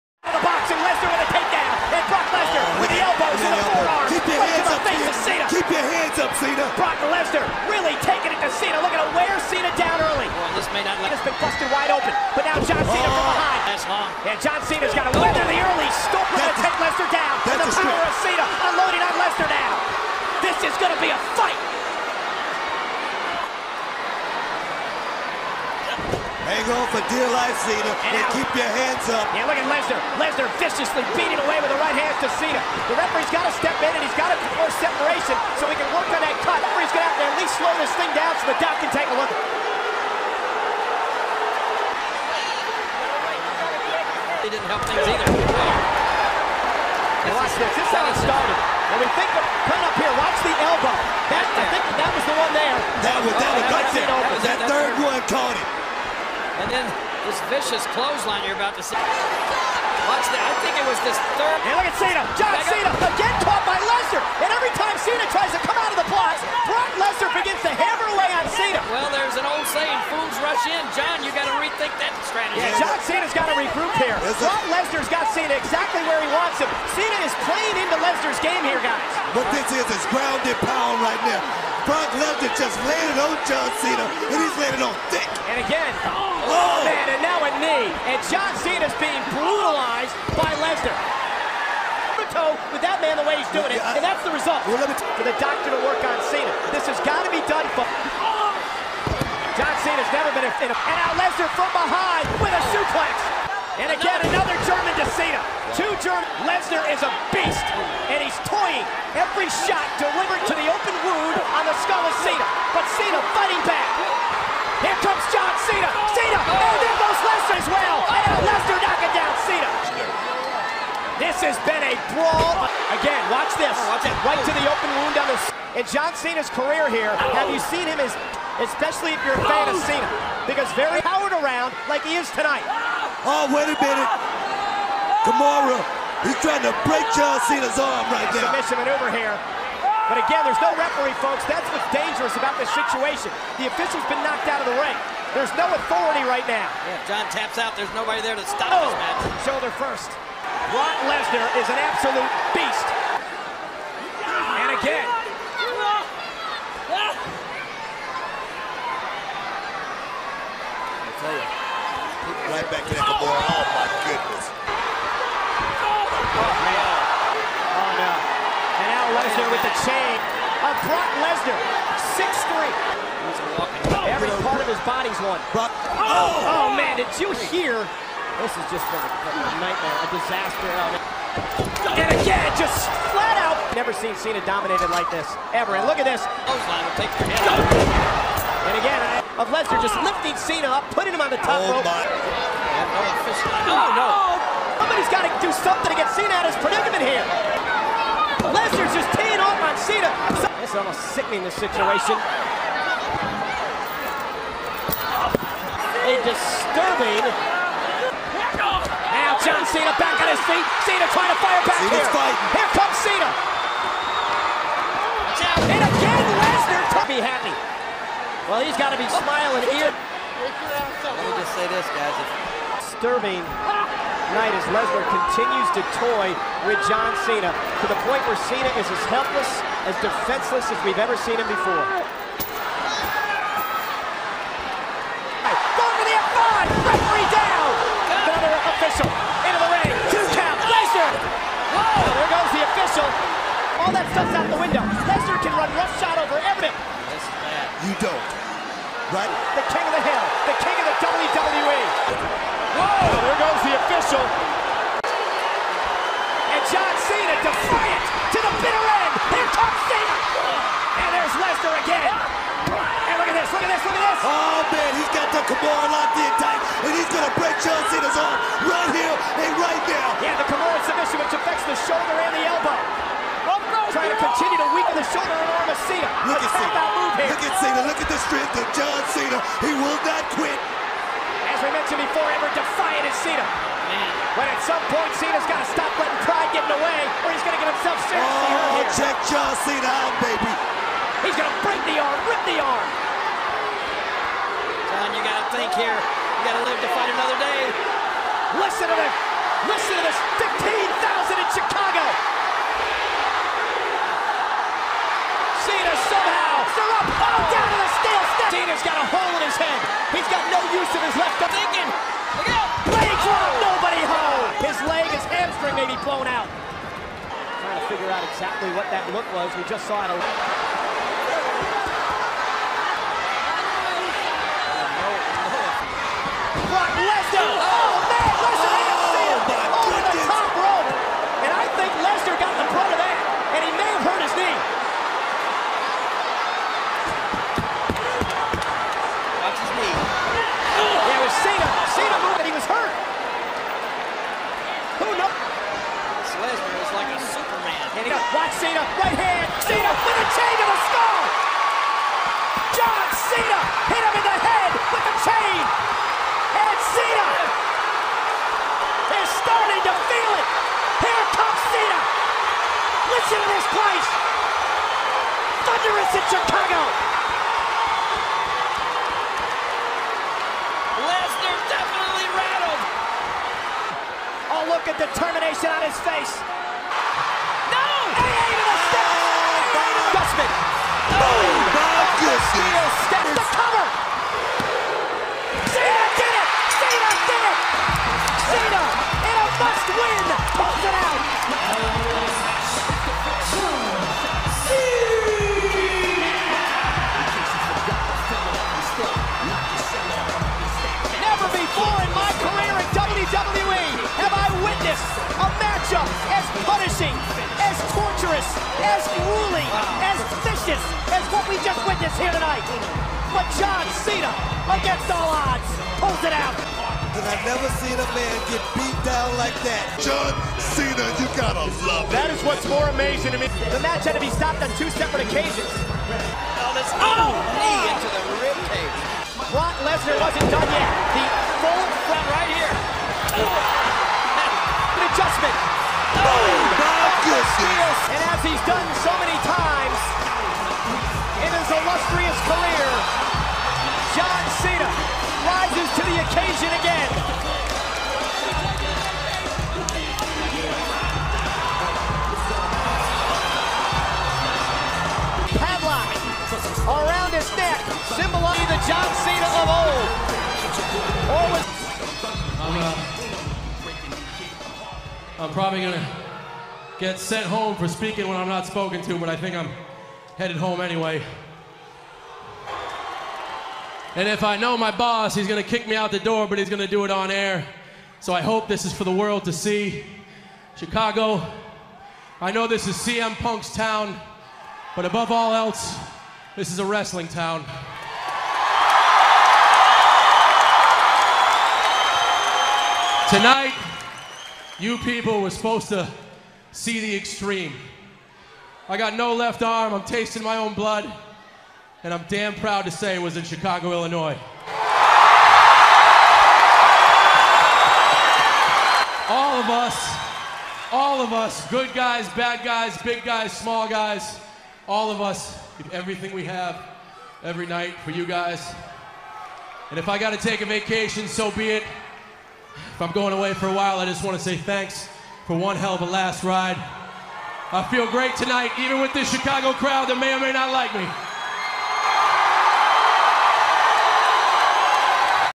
Full Match John Cena vs Brock Lesnar 2020 Fight Highlights
John Cena vs Brock Lesnar 2020 Wrestling Match.   See this bloody fight between Brock Lesnar and John Cena

Interesting Buys Just For You:
Wrestling Action Figures
Wrestling Shirts
Wrestling Shoes
Wrestling Belt
Wresrtling Toys